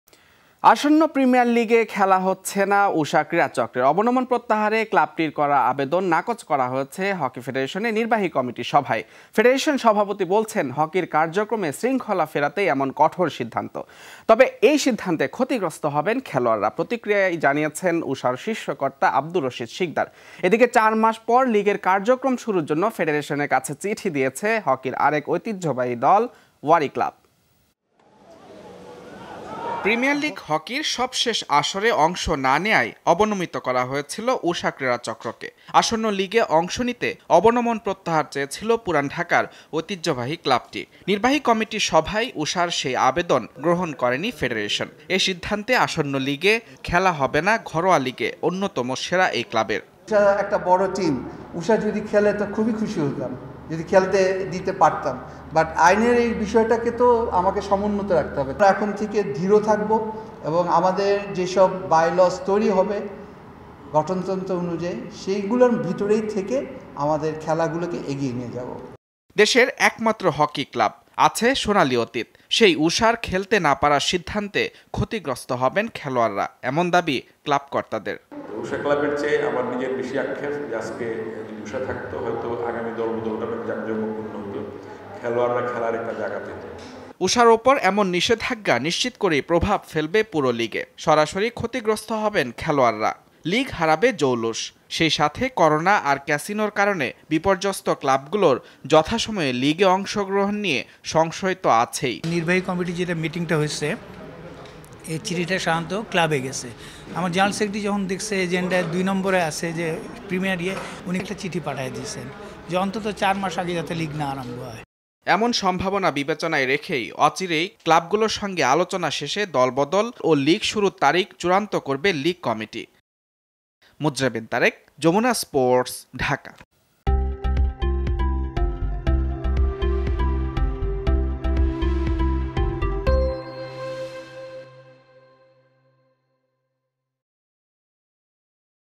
হকি ফেডারেশনের কঠিন সিদ্ধান্তে ক্ষতির মুখে খেলোয়াড়রা। Jamuna TV
আসন্ন প্রিমিয়ার হকি লিগে খেলা হচ্ছে না উষা ক্রীড়া চক্রের। অবনমন প্রত্যাহারে ক্লাবটির করা আবেদন নাকচ করা হয়েছে ফেডারেশনের নির্বাহী কমিটির সভায়। ফেডারেশন সভাপতি বলছেন, হকির কার্যক্রমে শৃঙ্খলা ফেরাতেই এমন কঠোর সিদ্ধান্ত। তবে এমন সিদ্ধান্তে ক্ষতিগ্রস্ত হবেন খেলোয়াড়রা, প্রতিক্রিয়ায় জানিয়েছেন উষার শীর্ষ কর্তা আব্দুর রশীদ শিকদার। এদিকে চার মাস পর লিগের কার্যক্রম শুরুর জন্য ফেডারেশনের কাছে চিঠি দিয়েছে হকির আরেক ঐতিহ্যবাহী দল ওয়ারি ক্লাব।